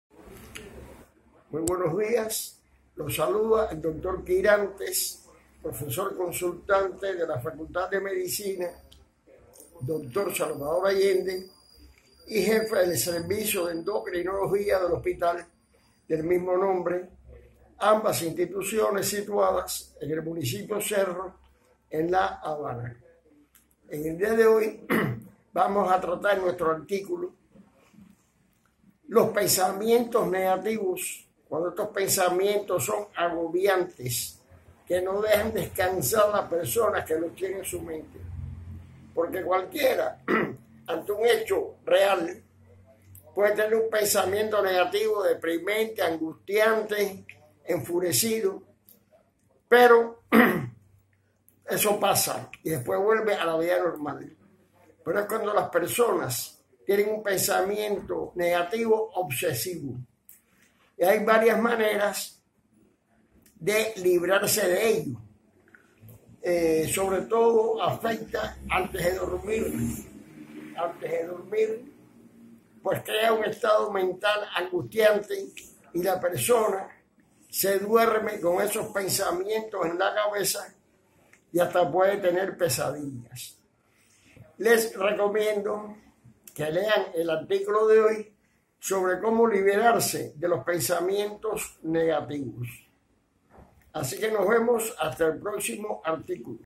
Cómo eliminar los pensamientos negativos
Es necesario mantener un estilo de vida saludable en todo sentido, pues las personas en buen estado físico tienen una predisposición menor a sufrir problemas con emociones inquietantes.